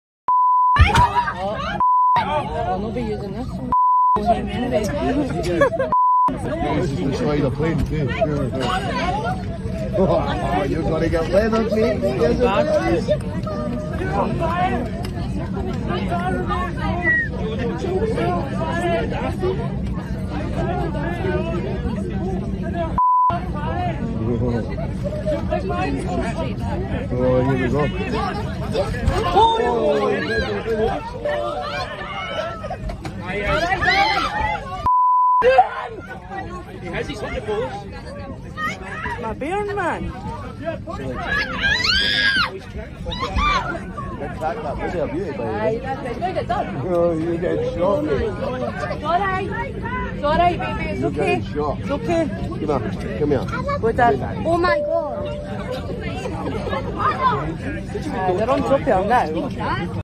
Sarhoş yolcu önce kendisini şikayet eden yolcuya ardından polise saldırdı
Edinburgh-Antalya seferli yolcu uçağında sarhoş bir yolcu, önce kendisini şikayet eden yolcuya yumruk attı, ardından araya giren polis memuruna saldırdı. Güçlükle kontrol altına alınan saldırgan yolcunun uçaktan indirilerek gözaltına alındığı öğrenildi.